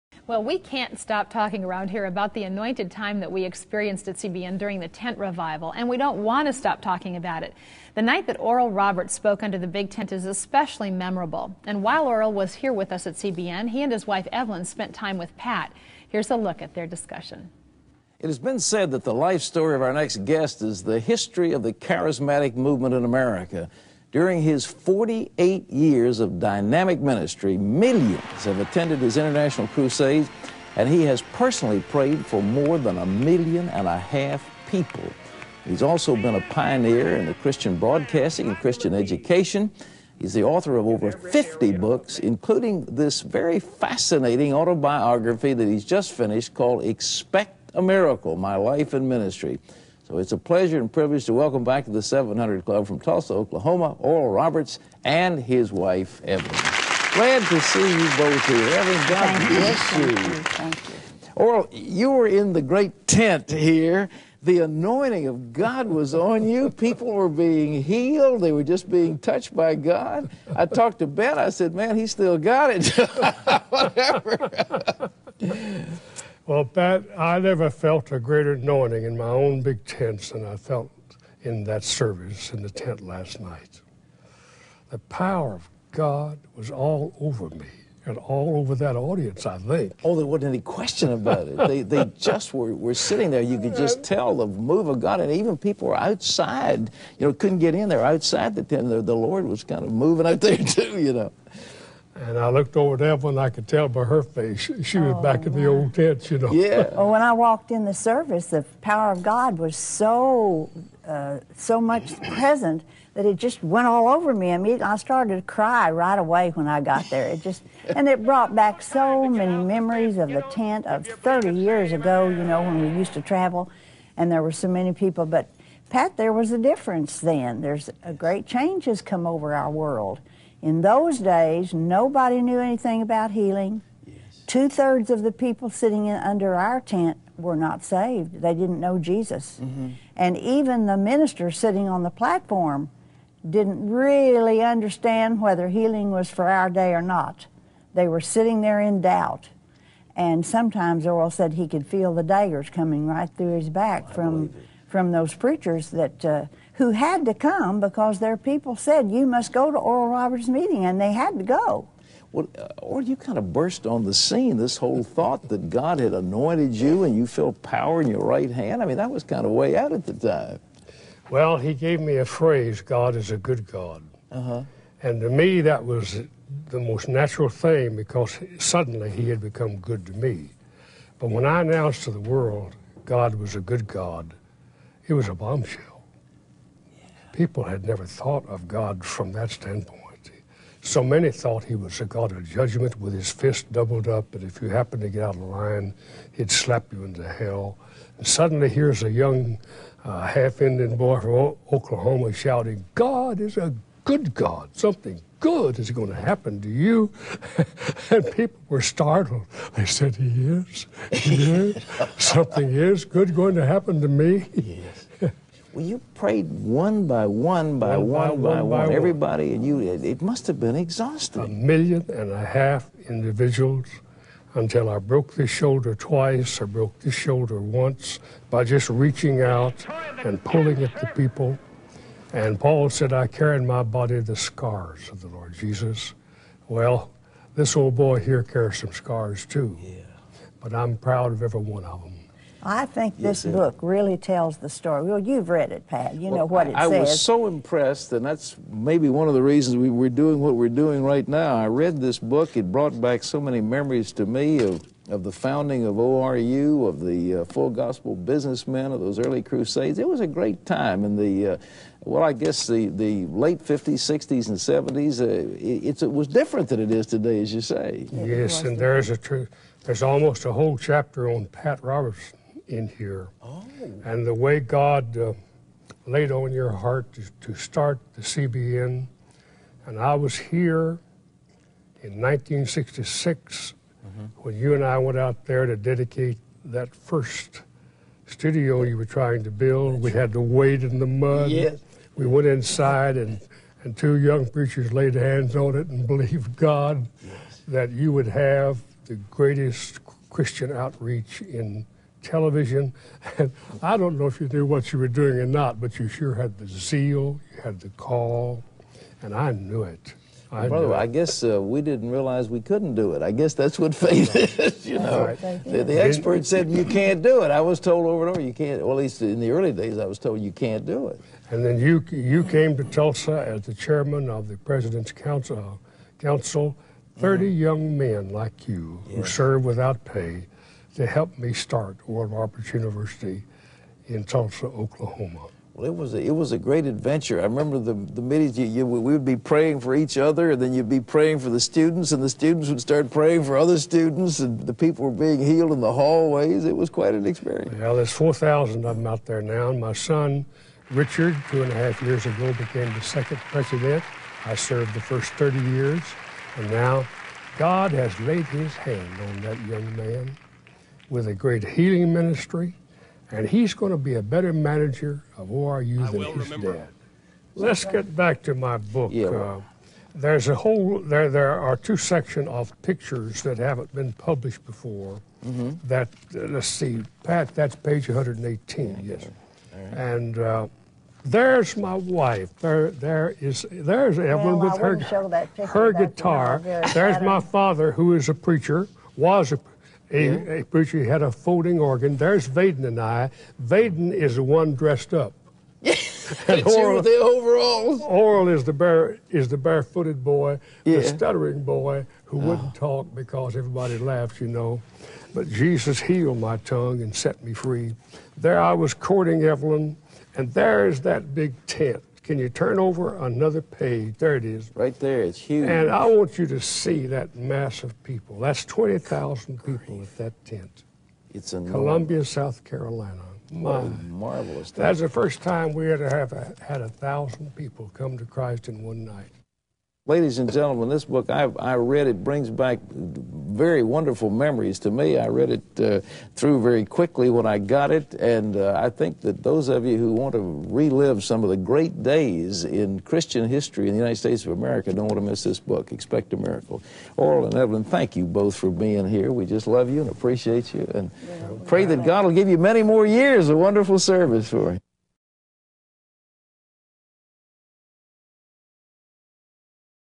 Oral and Evelyn Roberts November 6, 1995 - CBN.com
It has been said that the life story of Oral Roberts is the history of the charismatic movement in America.  Along with wife Evelyn, Oral sat down with Pat Robertson to reminisce about their 50 years of ministry... The Christian Broadcasting Network CBN